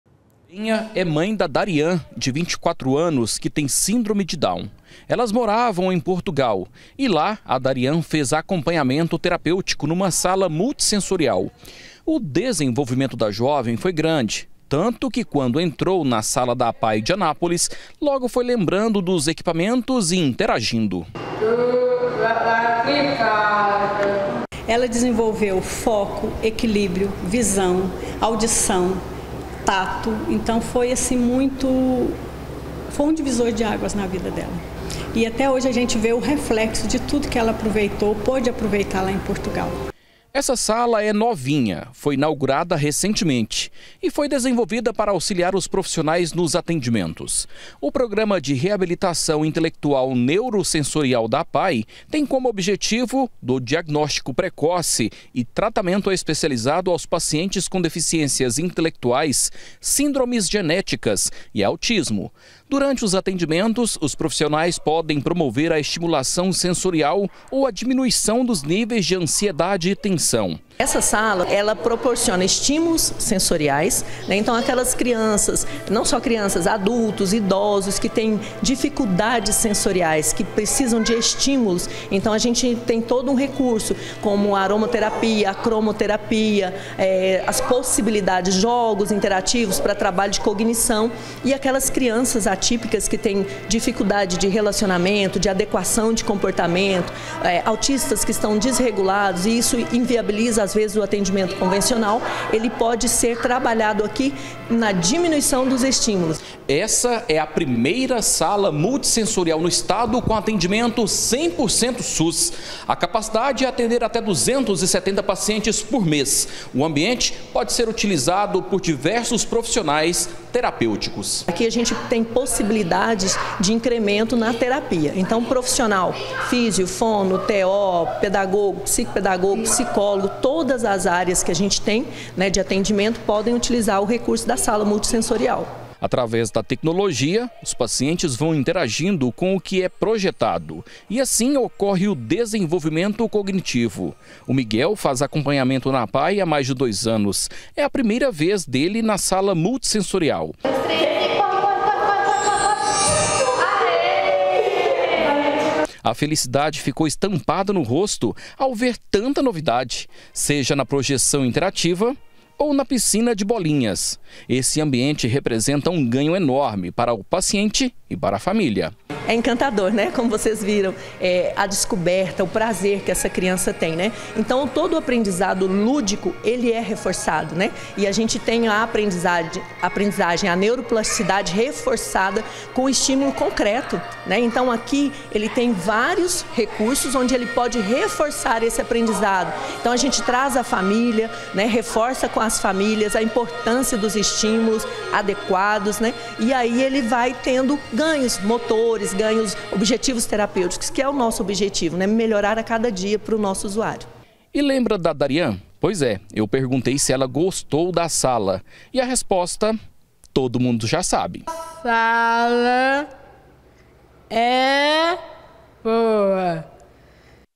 ATENDIMENTO GRATUITO: INAUGURADA PRIMEIRA SALA MULTISSENSORIAL DE GOIÁS
A APAE ANÁPOLIS INAUGUROU A PRIMEIRA SALA MULTISSENSORIAL DO ESTADO COM ATENDIMENTO PELO SUS. O ESPAÇO VAI SER USADOS PARA PACIENTES ATENDIDOS NO CENTRO DE REABILITAÇÃO INTELECTUAL E NEUROSSENSORIAL DA UNIDADE.